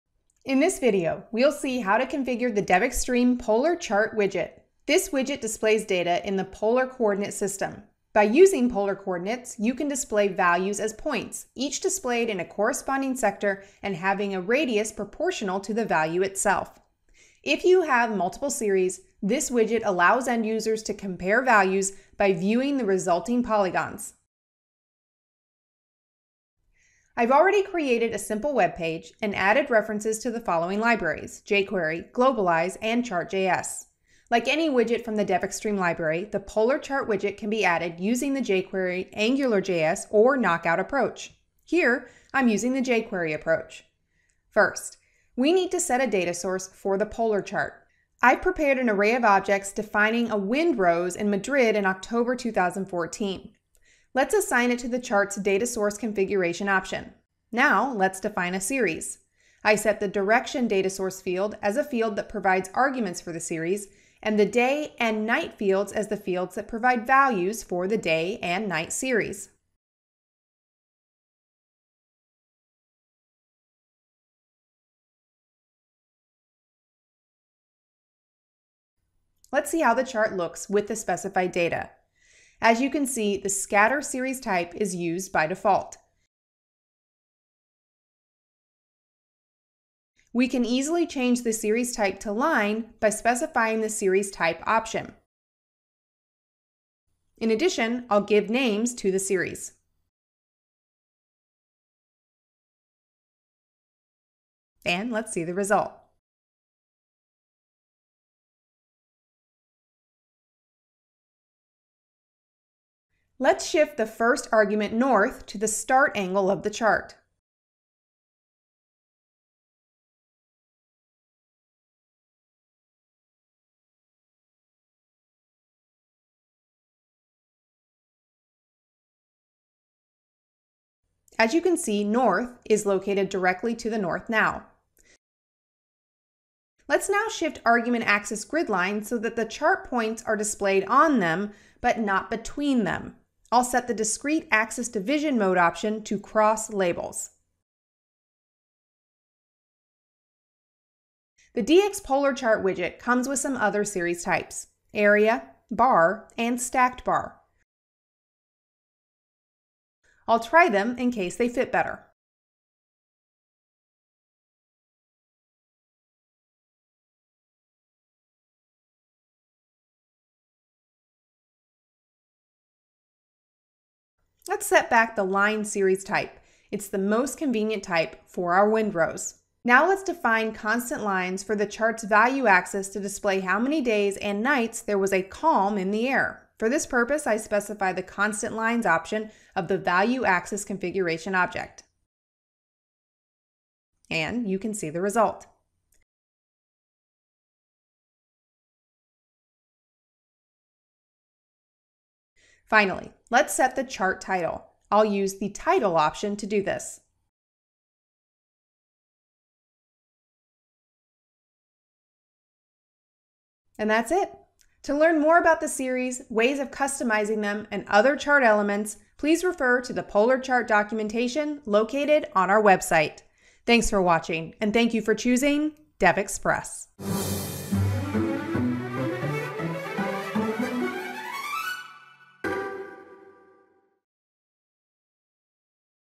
DevExpress DevExtreme: HTML5 Polar Chart
The dxPolarChart widget displays data in the polar coordinate system. By using polar coordinates, you can display values as points - each displayed in a corresponding sector and having a radius proportional to the value itself. If you have multiple series, this widget allows end-users to compare values by viewing the resulting polygons.